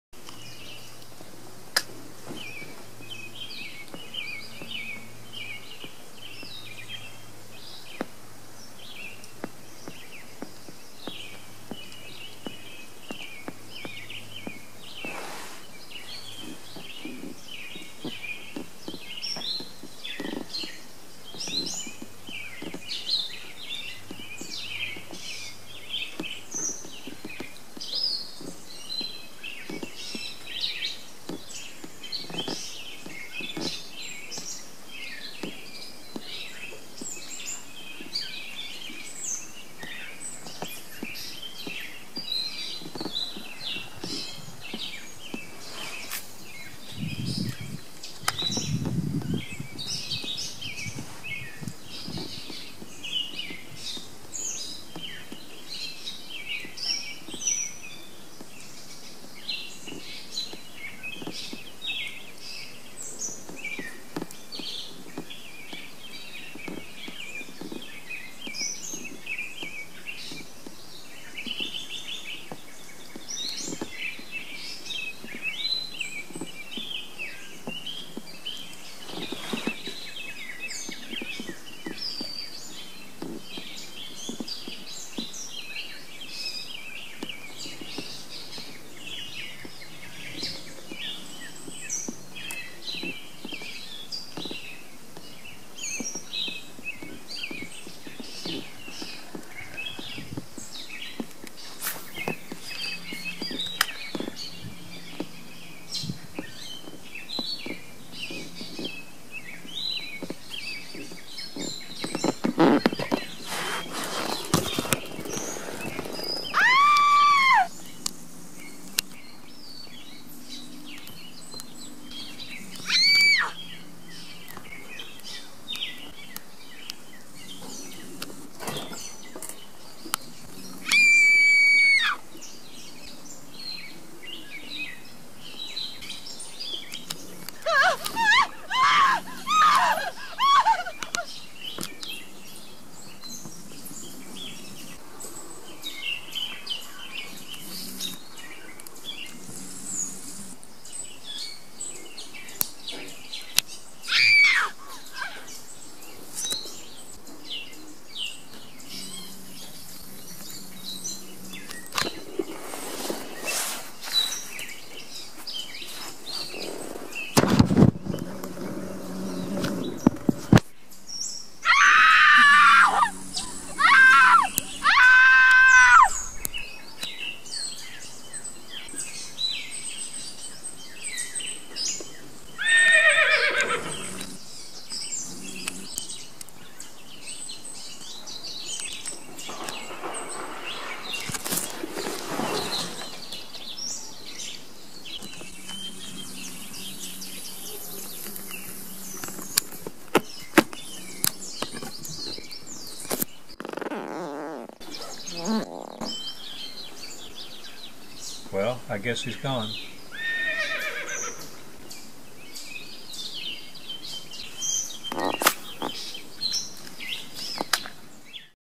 Prof. Washboard fishing companion Cove May/15
Had the camera on a holder just filming whatever. Sorry for the comments. Neighbor surprised me.